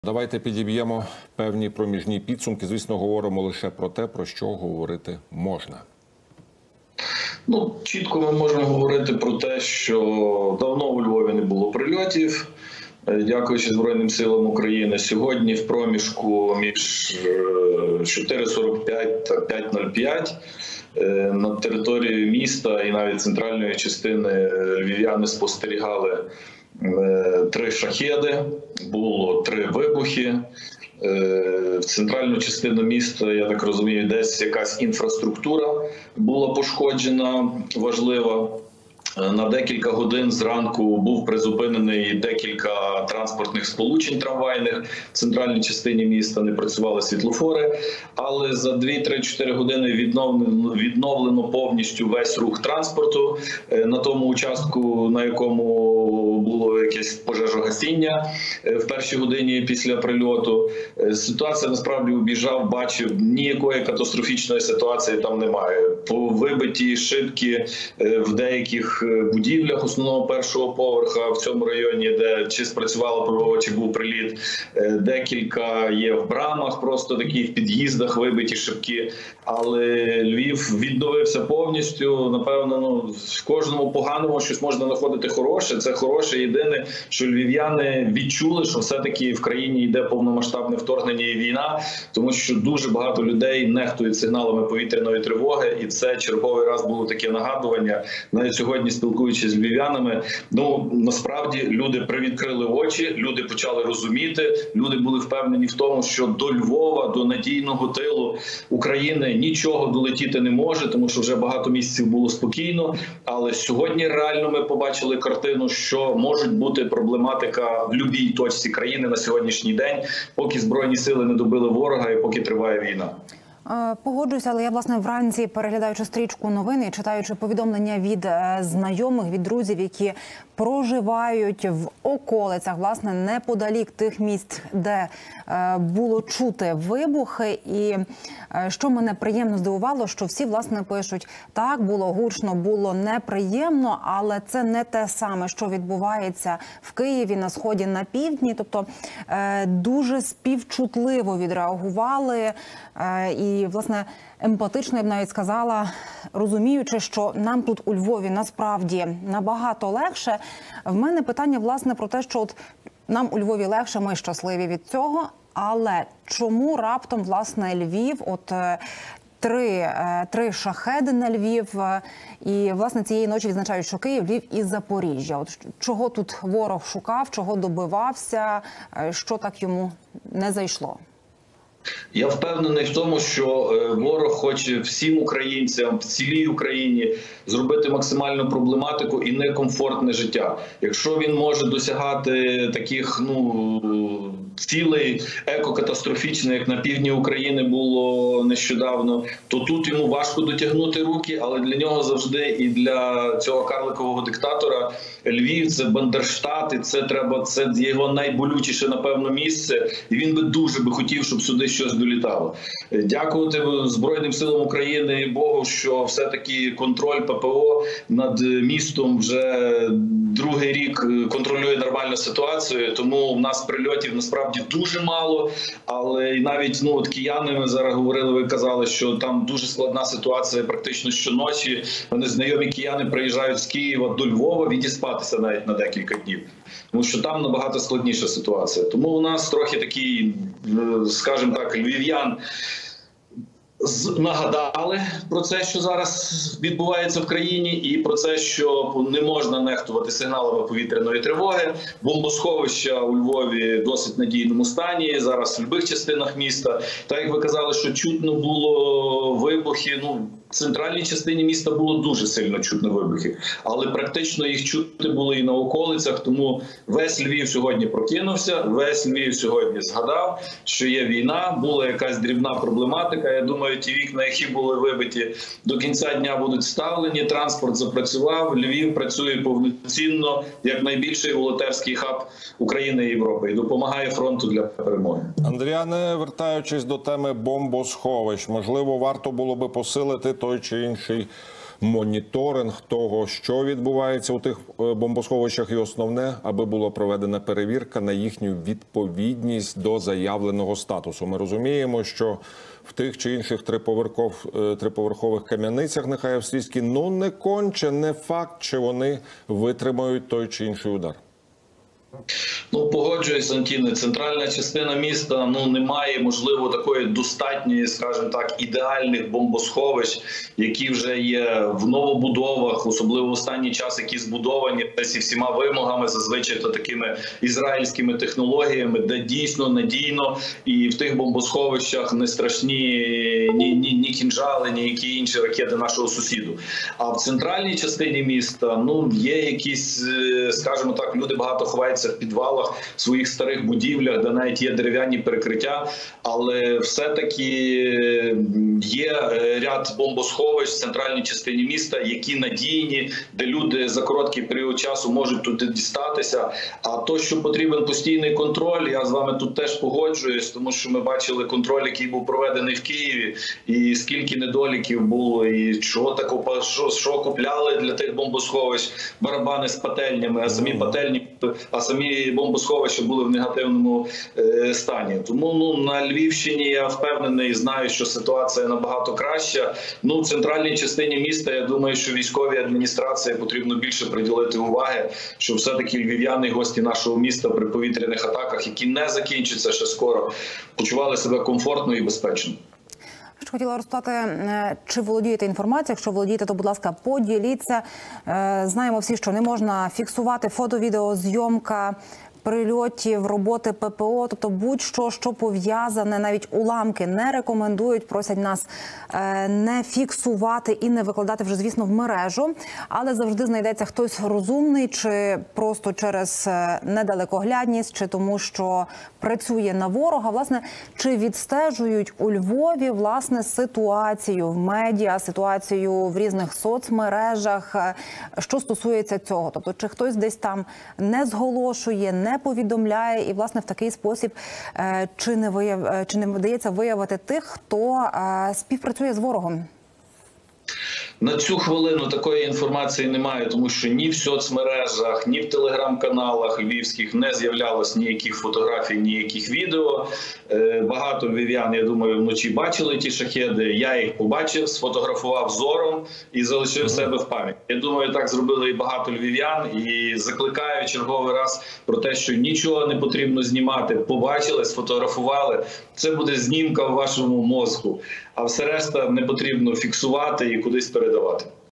Львів зазнав ворожої повітряної атаки: про наслідки та ситуацію в місті інформує Андріян Гутник
Рано вранці 20 червня Львів прокинувся від вибухів. Що це було та які наслідки — інформує Андріян Гутник, член центрального штабу опору Львова, депутат Львівської міської ради,  волонтер "Легіону Свободи".
З етеру телеканалу "Еспресо" 20 червня 2023 року.